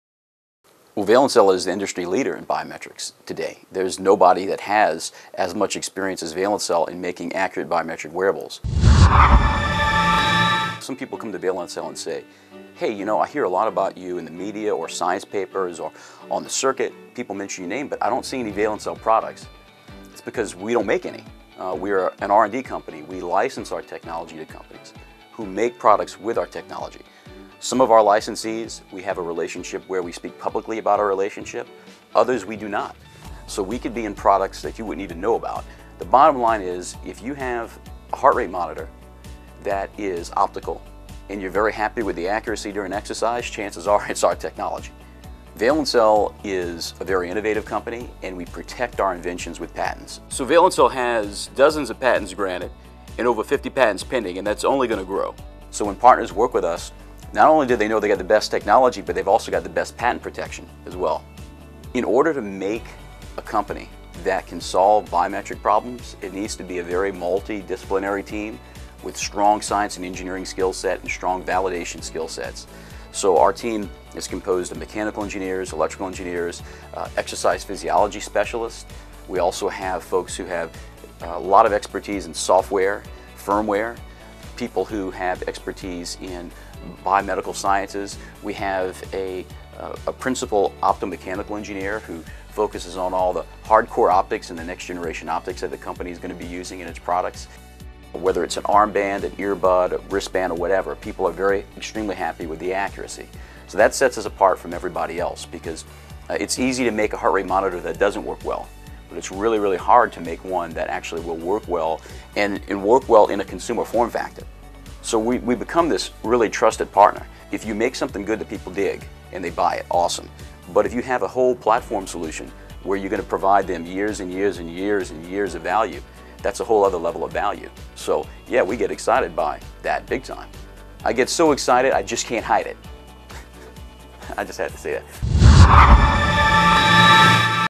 Leading biometric technology for wearables
Dr. LeBoeuf explains the Valencell value.